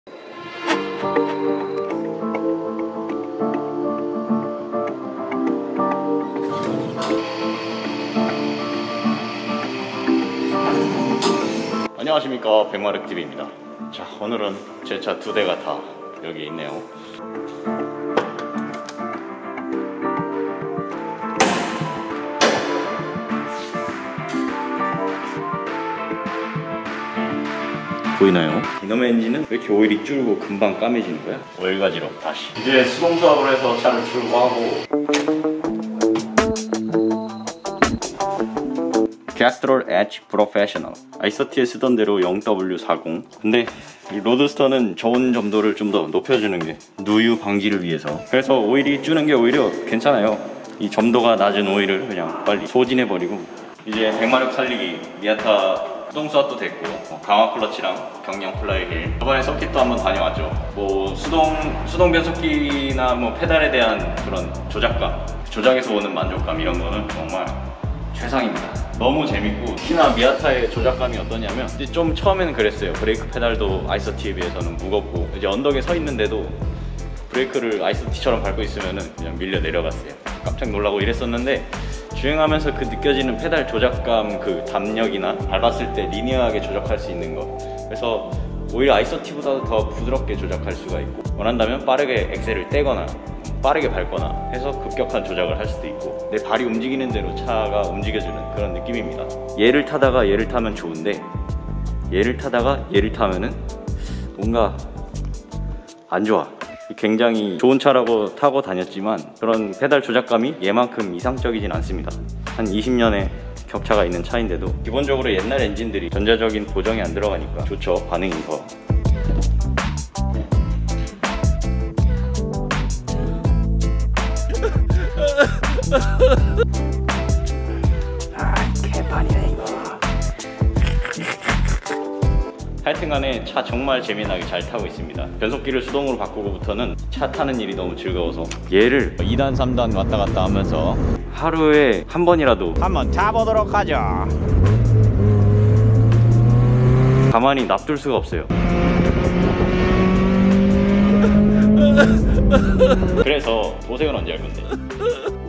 NA 1세대 미아타 조작감이란...
미아타 조작감 지리죠모 1년간 30일정도밖에 못타봤지만...간럏간 후기입니다 ㅎㄷㄷㄷㄷ